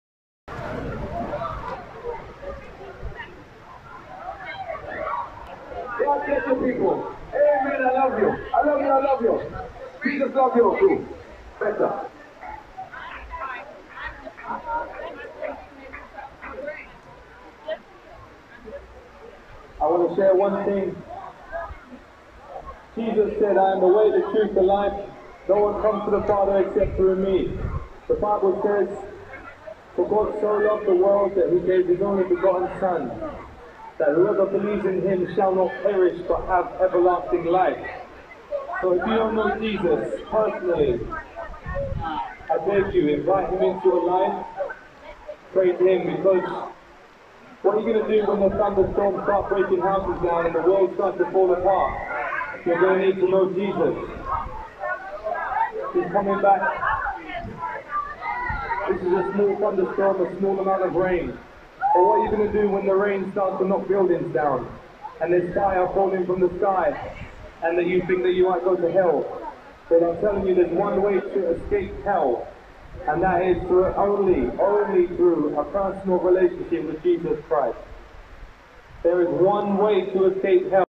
Huge rain and street preacher's (Croydon, South London)
They were lucky everyone had to listen to it.